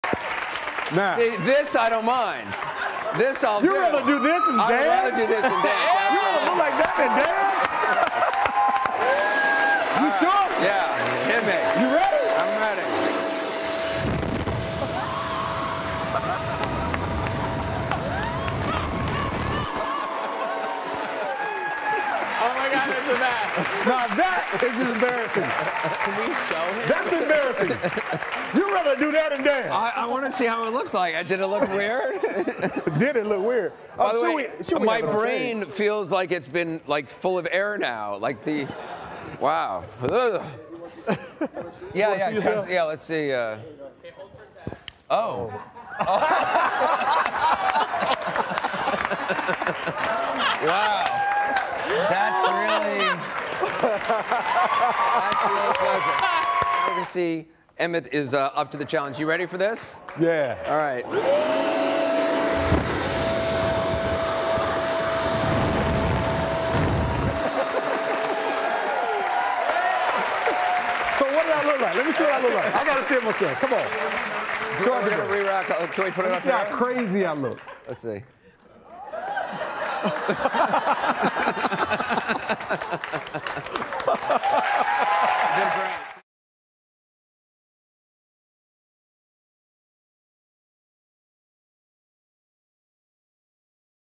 Slow Motion with a Leaf Blower
On today's show, Anderson and Emmitt bravely stood in front of a leaf blower and watched their faces ripple in the wind.